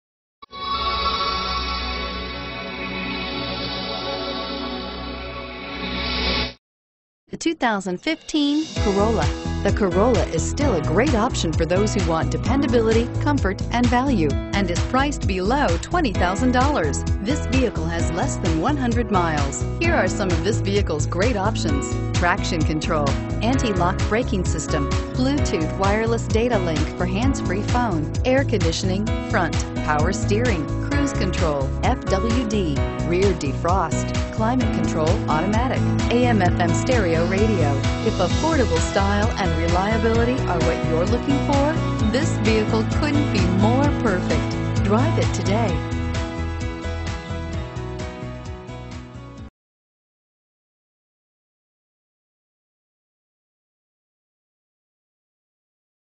2015 Toyota Corolla Union City GA C331437
2015 Toyota Corolla LE

Looking for the right car? Today could be your lucky day. With 10 miles, this Super White, 2015 Toyota Corolla equipped with Automatic transmission could be yours. Request more information and set up a test drive right away.